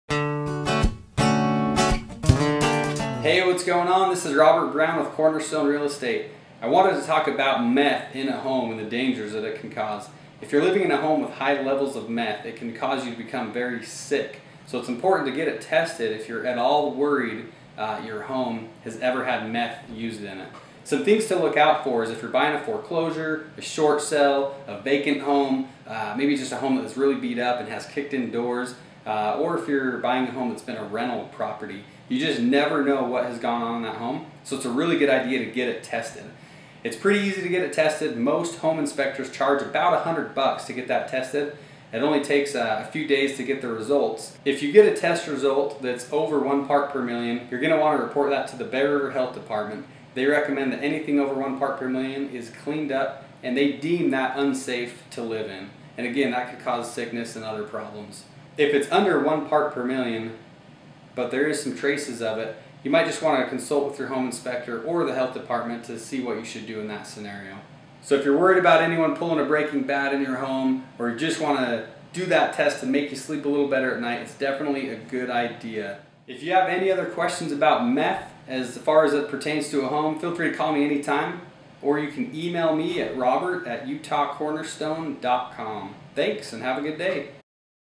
How to Make Sure You Don't Buy a Meth Home - Real Estate in Logan Utah - Episode #47
How to test a home for meth, when buying in Northern Utah.

Robert J. Brown
Broker - Owner
Cornerstone Real Estate